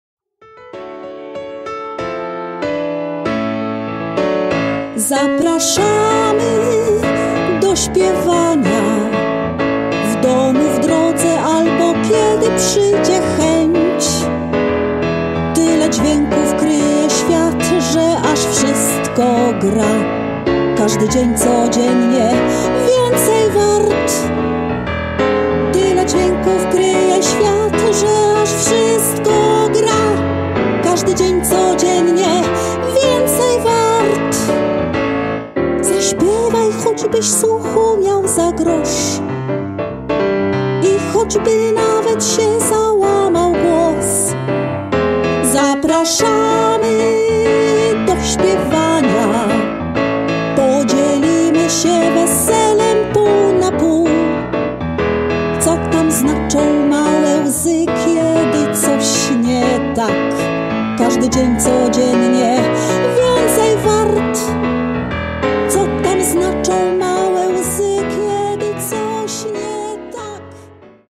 Wspólne śpiewanie po raz ósmy
"Zapraszamy do śpiewania" to cykl spotkań mających na celu przypomnienie największych polskich przebojów minionej epoki (lata dwudzieste do początków lat dziewięćdziesiątych ubiegłego wieku), wspólne ich zaśpiewanie, porozmawianie o autorach, kompozytorach i wykonawcach.